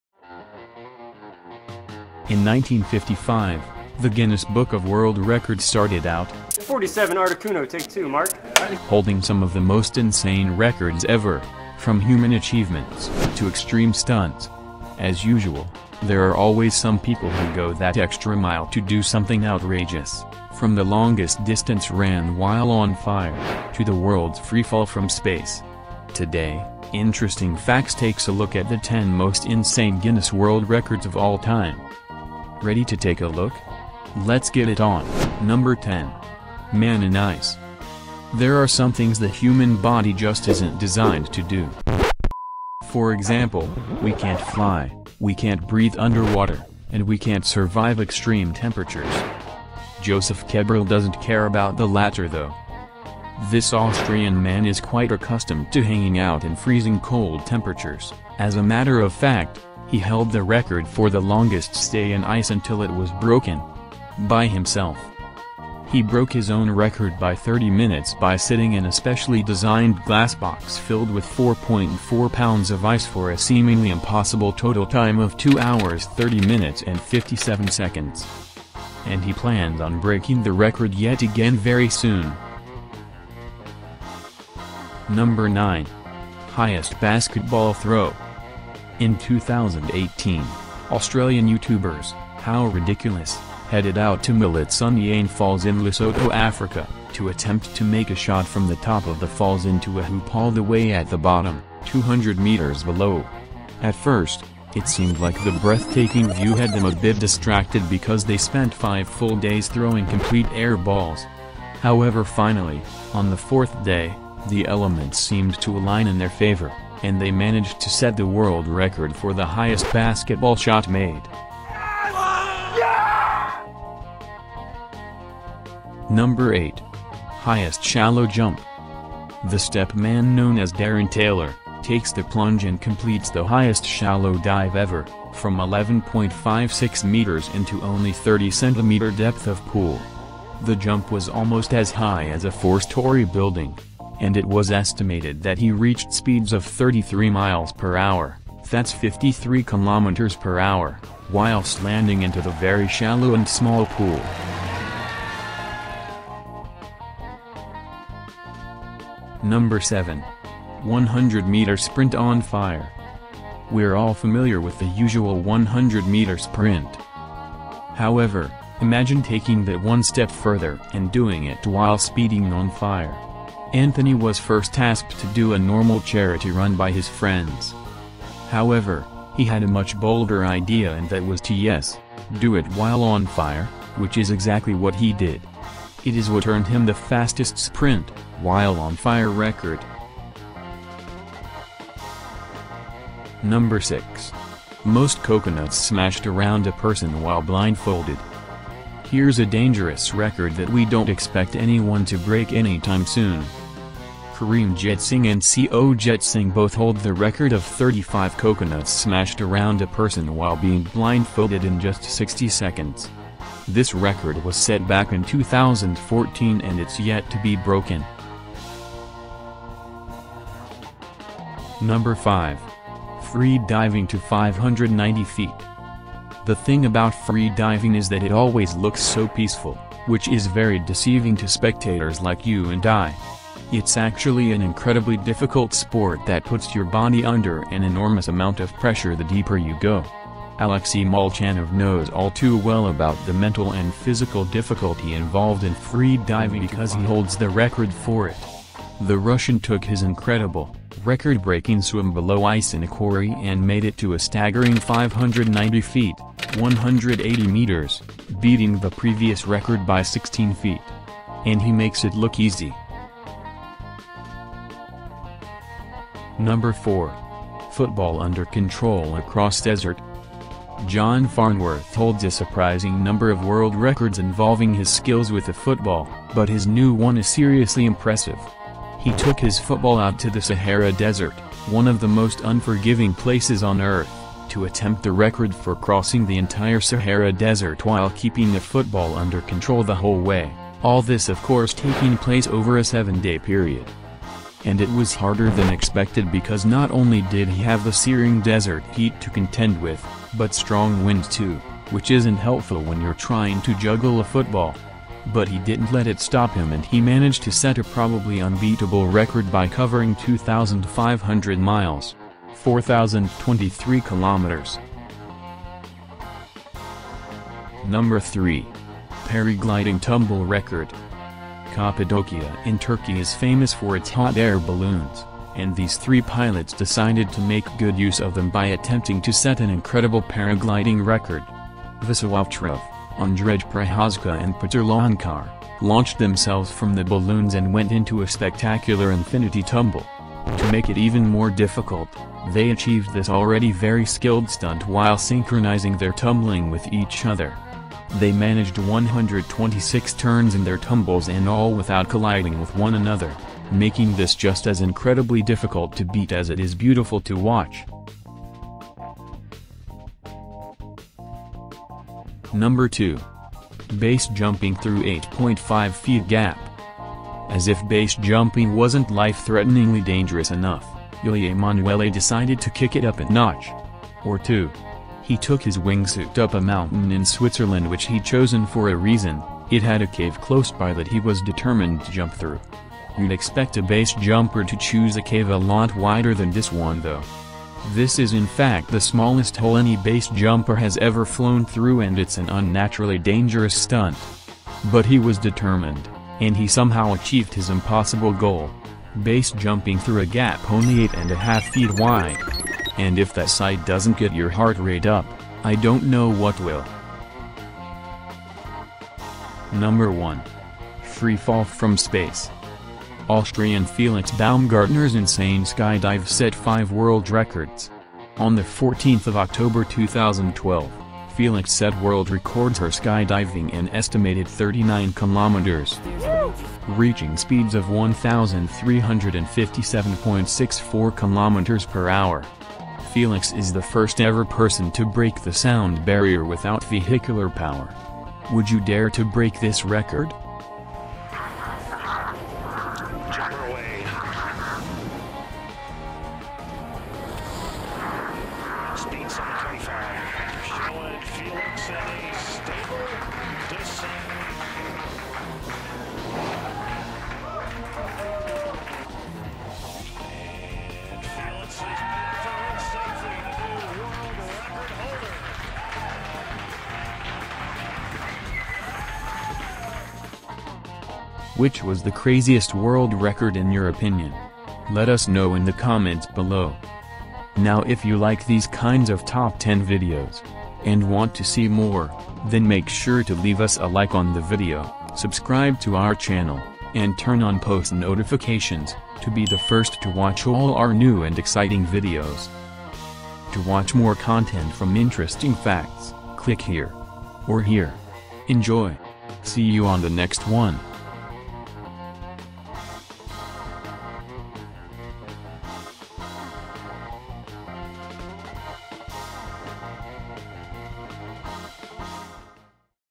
10 Most Insane Guinness World Records of All Time
10 Most Insane Guinness World Records of All Time

00:00 Intro
00:37 Number 10. Man in Ice.
There are some things the human body just isn’t designed to do.
For example, we can’t fly, we can’t breathe underwater, and we can’t survive extreme temperatures.
Josef Keberl doesn’t care about the latter though.
This Austrian man is quite accustomed to hanging out in freezing cold temperatures, as a matter of fact, he held the record for the longest stay in ice until it was broken… by himself.

01:27 Number 9. Highest Basketball Throw.
In 2018, Australian YouTubers, “How Ridiculous”, headed out to Maletsunyane Falls in Lesotho Africa, to attempt to make a shot from the top of the falls into a hoop all the way at the bottom, 200 meters below. At first, it seemed like the breathtaking view had them a bit distracted because they spent 5 full days throwing complete air balls.

02:08 Number 8. Highest Shallow Jump.
The step man known as Darren Taylor, takes the plunge and completes the highest shallow dive ever, from eleven point five six meters into only 30 Centimeter depth of pool.  The jump was almost as high as a four-story building, and it was estimated that he reached speeds of 33 miles per hour, that’s 53 kilometers per hour, whilst landing into the very shallow and small pool.

02:39 Number 7. 100-Meter Sprint on Fire.
We're all familiar with the usual 100-meter sprint.
However, imagine taking that one step further and doing it while speeding on fire.  Anthony was first asked to do a normal charity run by his friends however, he had a much bolder idea and that was to yes, do it while on fire, which is exactly what he did.  It is what earned him the fastest sprint, while on fire record.

03:13 Number 6. Most Coconuts Smashed Around A Person While Blindfolded.
Here's a dangerous record that we don't expect anyone to break anytime soon. Karim Jet Singh and Co Jet Singh, both hold the record of 35 coconuts smashed around a person while being blindfolded in just 60 seconds.  This record was set back in 2014 and it's yet to be broken.

03:41 Number 5. Free Diving to 590 Feet.
The thing about free diving is that it always looks so peaceful, which is very deceiving to spectators like you and I. It’s actually an incredibly difficult sport that puts your body under an enormous amount of pressure the deeper you go. Alexey Molchanov knows all too well about the mental and physical difficulty involved in freediving because he holds the record for it.

04:26 Number 4. Football Under Control Across Desert.
John Farnworth holds a surprising number of world records involving his skills with a football, but his new one is seriously impressive.  He took his football out to the Sahara Desert, one of the most unforgiving places on earth, to attempt the record for crossing the entire Sahara Desert while keeping a football under control the whole way, all this of course taking place over a seven-day period.

05:16 Number 3. Paragliding Tumble Record.
Cappadocia in Turkey is famous for its hot air balloons, and these three pilots decided to make good use of them by attempting to set an incredible paragliding record. Veso Ovcharov, Ondrej Prochazka and Petar Lončar, launched themselves from the balloons and went into a spectacular infinity tumble. To make it even more difficult, they achieved this already very skilled stunt while synchronizing their tumbling with each other. They managed 126 turns in their tumbles and all without colliding with one another, making this just as incredibly difficult to beat as it is beautiful to watch.

06:02 Number 2. BASE Jumping through 8.5 Feet Gap.
As if base jumping wasn’t life-threateningly dangerous enough, Uli Emanuele decided to kick it up a notch… or two. He took his wingsuit up a mountain in Switzerland which he’d chosen for a reason: it had a cave close by that he was determined to jump through. You’d expect a base jumper to choose a cave a lot wider than this one though. This is in fact the smallest hole any base jumper has ever flown through and it’s an unnaturally dangerous stunt. But he was determined, and he somehow achieved his impossible goal: base jumping through a gap only 8 and a half feet wide. And if that sight doesn’t get your heart rate up, I don’t know what will.

06:49 Number 1. Free Fall from Space.
Austrian Felix Baumgartner's insane skydive set 5 world records.  On the 14th of October 2012, Felix set world records her skydiving an estimated 39 Kilometers, reaching speeds of one thousand three hundred and fifty-seven point six four kilometers per hour. Felix is the first-ever person to break the sound barrier without vehicular power.  Would you dare to break this record?